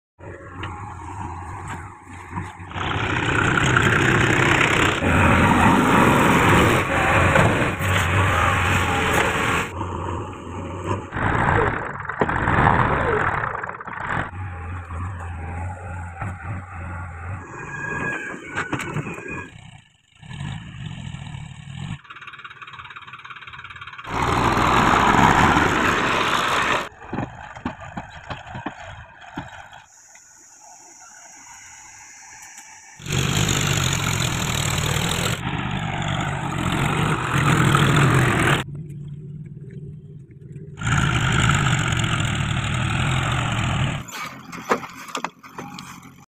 JCB 3dx loading with tractors Massey Ferguson and John Deere | Short Videos
Tractors Videos,
Trucks Videos,
All  Heavy Vehicles Working Videos,
All Types Of Village Vlogs.